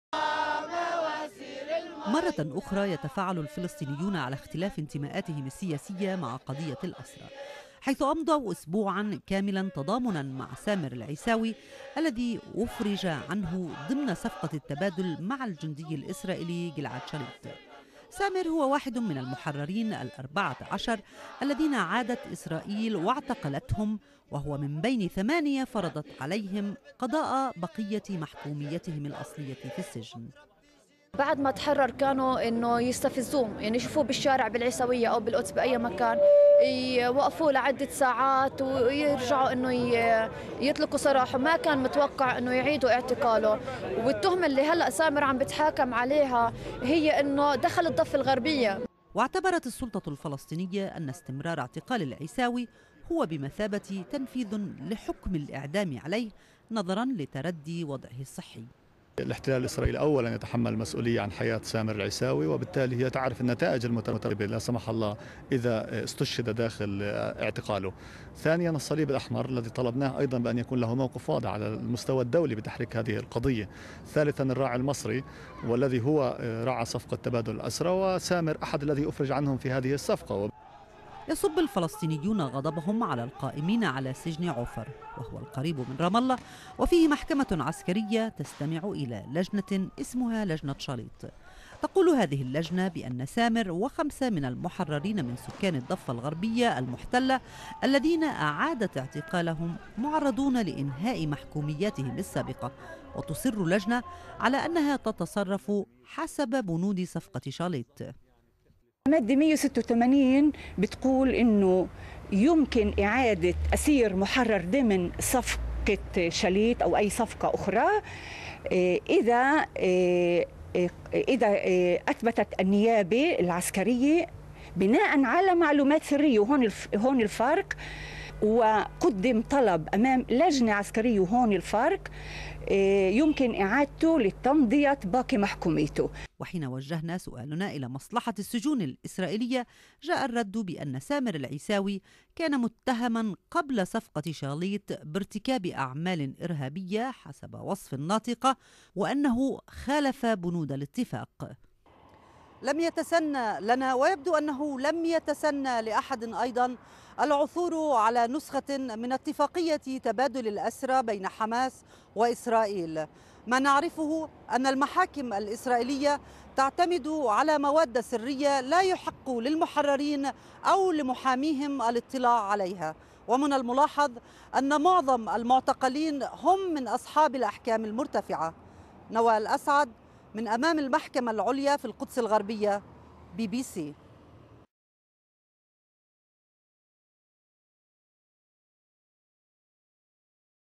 اعتصامات فلسطينية لإطلاق سراح المعتقل سامر العيساوي
تتواصل الاحتجاجات في الأراضي الفلسطينية المحتلة وإسرائيل تضامنا مع المعتقلين المضربين فيما يعرف بمعركة الأمعاء الخاوية للضغط على إدارة السجون الإسرائيلية لتحسين معيشتهم.